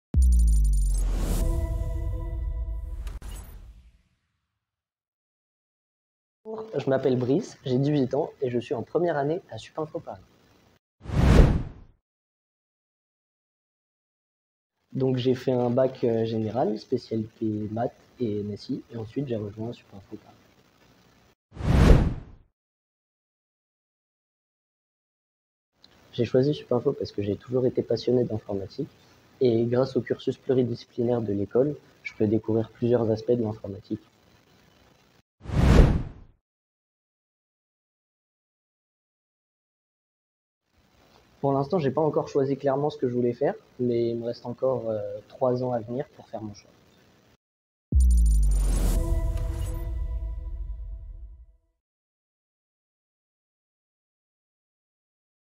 Portrait d'Étudiant : Brice Aubineau
"Grâce au cursus pluridisciplinaire de l'école je peux découvrir plusieurs aspects de l'informatique." 💬

Découvrez le portrait de Brice Aubineau, étudiant en première année de sur le campus de Paris !